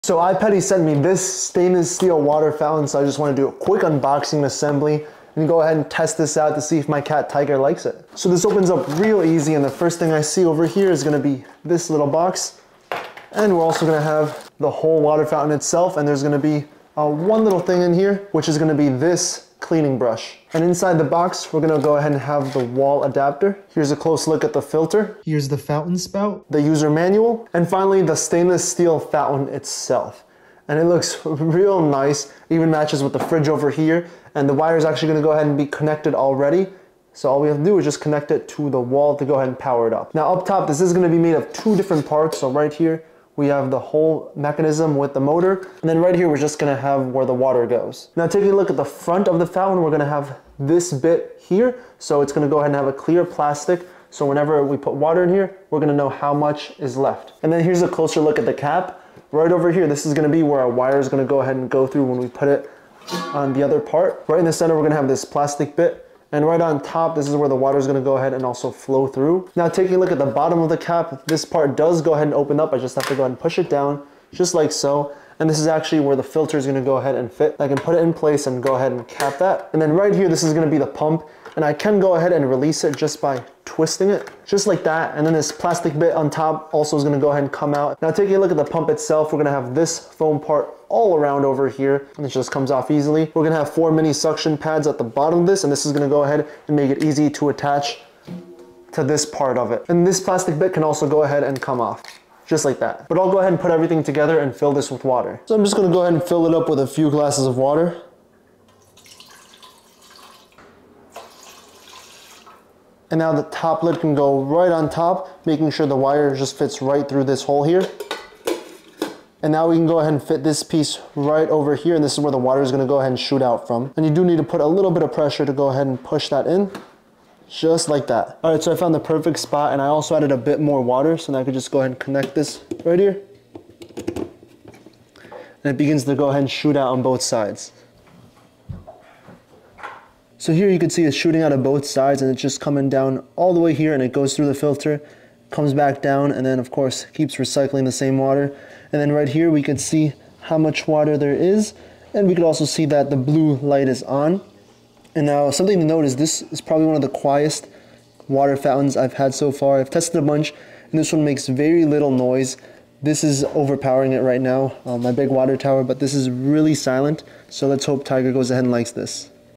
iPettie Rotunda Cat Water Fountain Stainless Steel Review
In this video, I review the iPettie Rotunda Cat Water Fountain Stainless Steel.